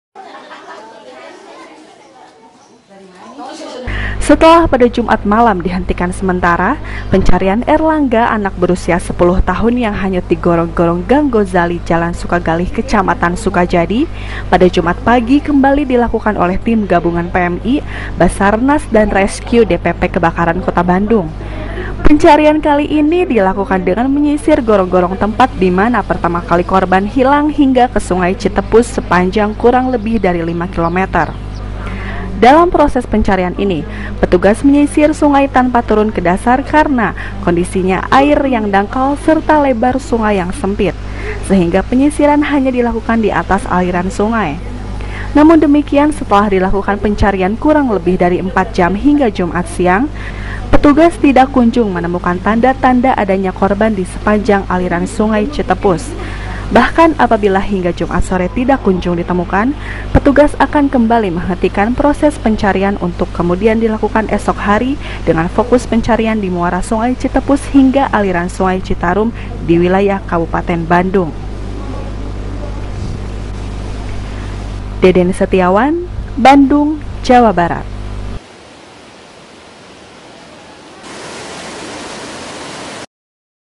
PENCARIAN KORBAN HANYUT DI GORONG GORONG BELUM MEMBUAHKAN HASIL
Terima Kasih Telah Melihat Video Channel Kami.